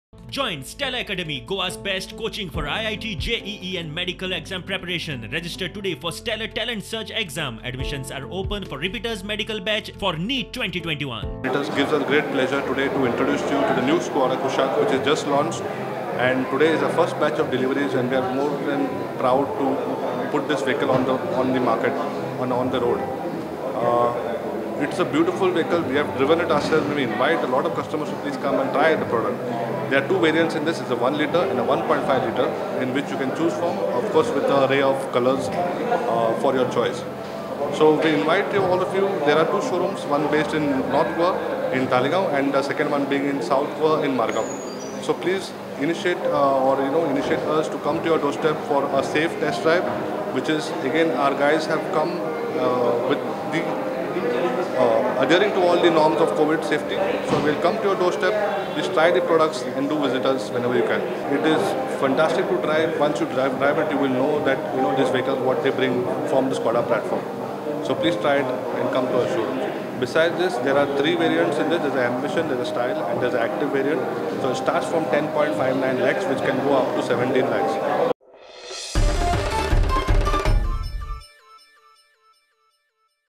FIRST BATCH OF SKODA KUSHAQ DELIVERED TO ITS CUSTOMERS IN GOA
Skoda Kushaq has finally arrived in Goa. Priority Auto delivered the first batch of the Kushaq to its customers. The 5 seater car was launched by Skoda in June.